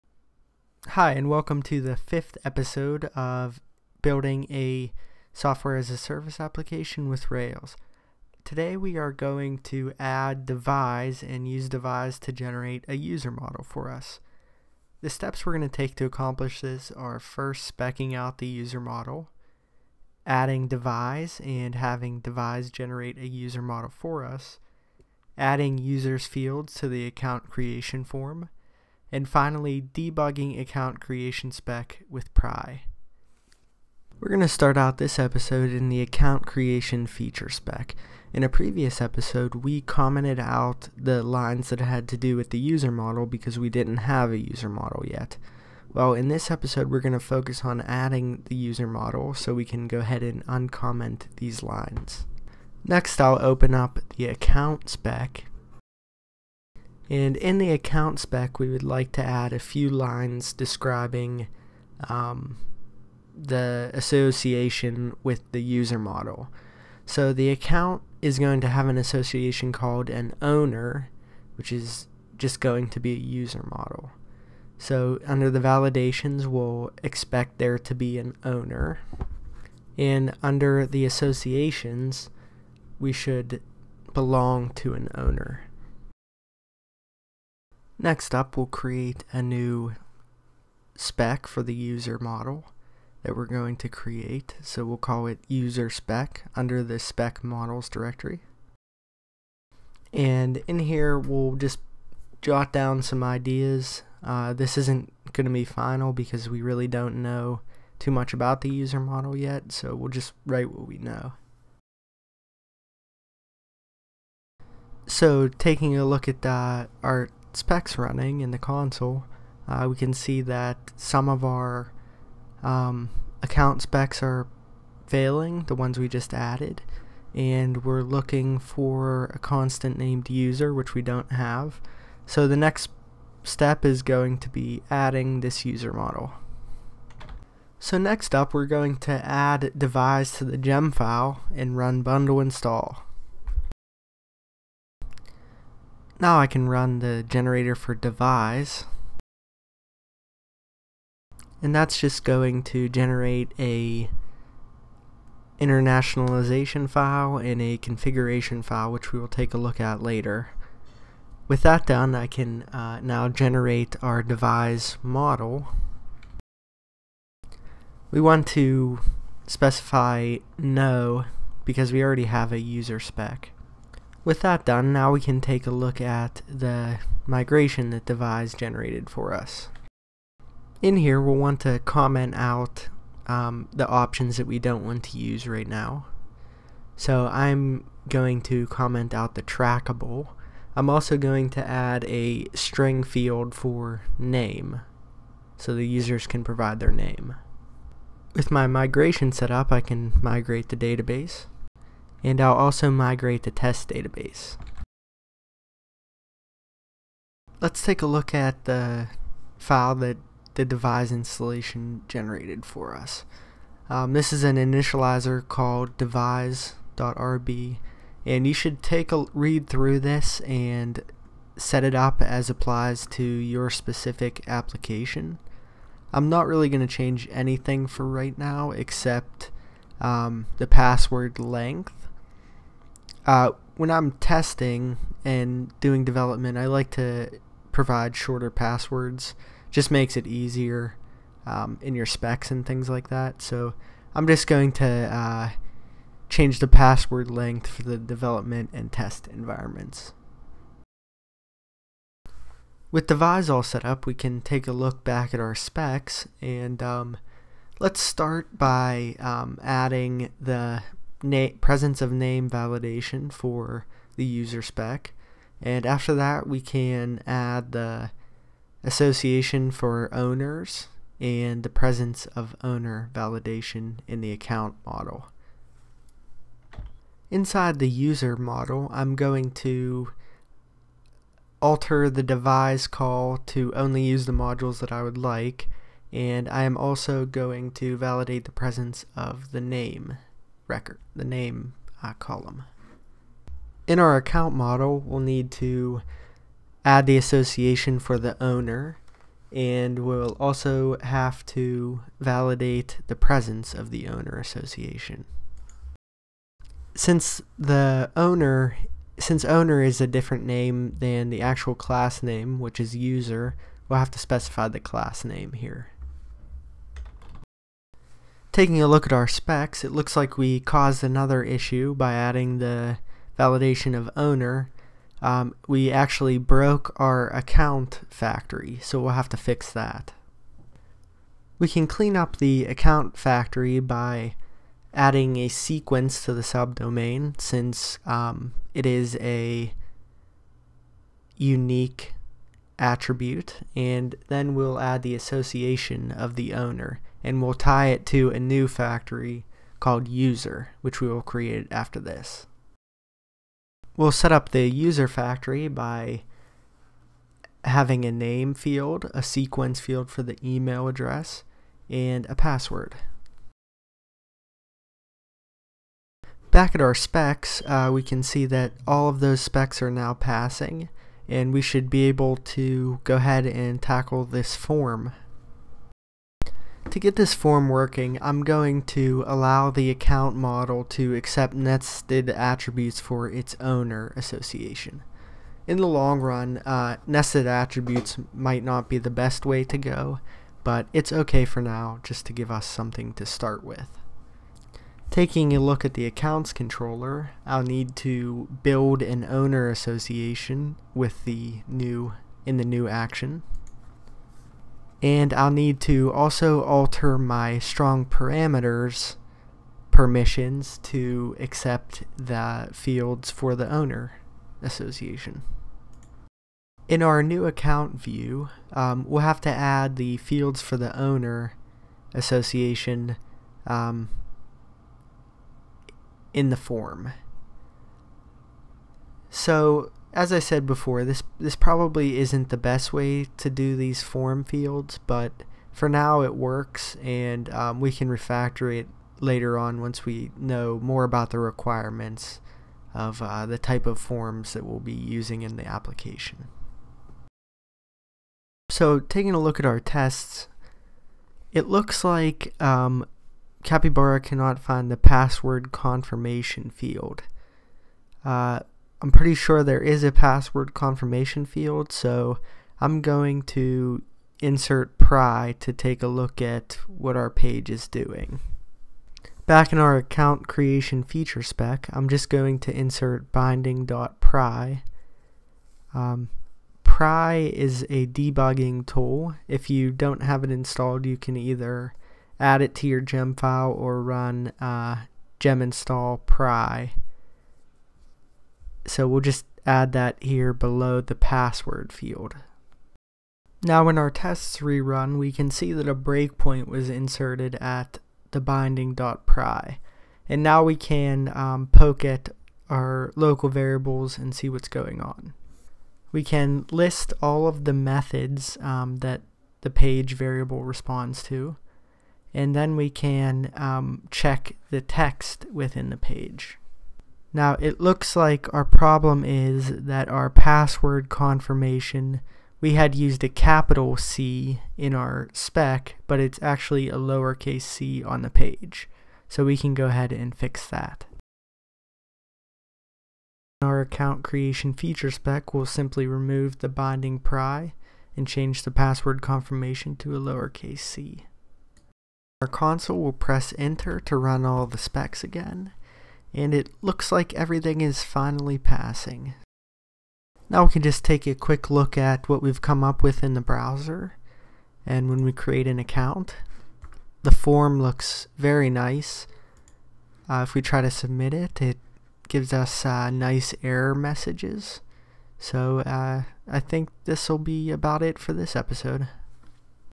Building a SaaS App with Rails #05 - Add Devise & User Model
In this series, I will show how to build a multi tenant SaaS (similar to Harvest or Basecamp) with Ruby on Rails and Postgresql.

In Episode #5, we will:
- Spec out User model
- Add Devise & generate User model
- Add User fields to Account creation form
- Debug Account creation spec with Pry

If you have any comments or feedback please let me know.